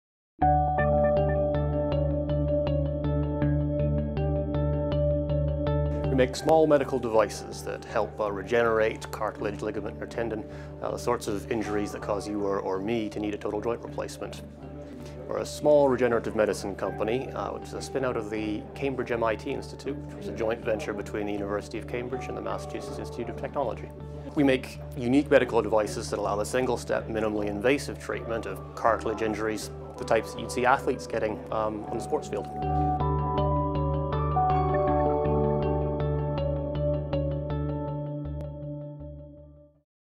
2008 ACES Spin-out Award Winner - Orthomimetics
ACADEMIC ENTERPRISE AWARDS EUROPE Andew Lynn of Orthomimetics discusses the medical device company that designs, develops and manufactures cutting-edge treatments for the repair of damaged joints following sports injuries and other orthopaedic trauma. Orthomimetics is a spin-out from the joint Cambridge/MIT institute.